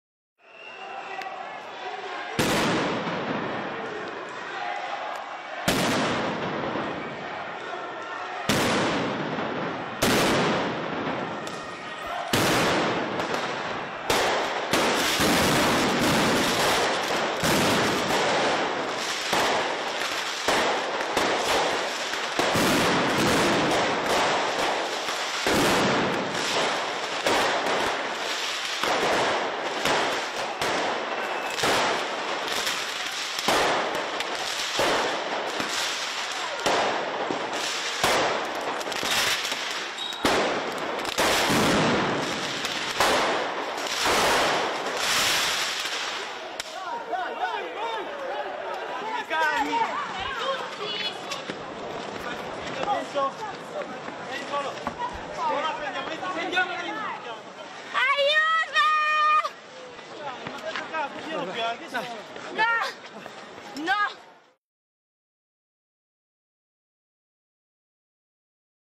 Italian Police, Anti-G7 Protesters Clash In Turin
September 30, 2017 - Watch Live YouTube Twitter LiveLeak Facebook Google+ Instagram Dailymotion